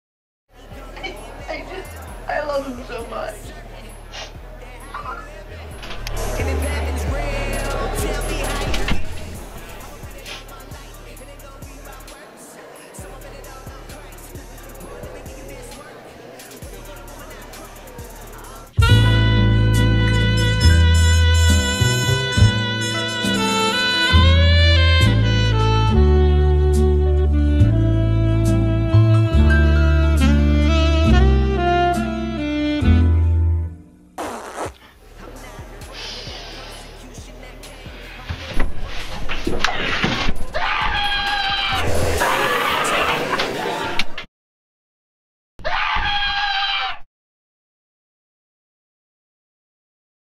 A Party Suprise
This Video is about a "girl" who is madly in Love with this guy. but the guy has other feelings!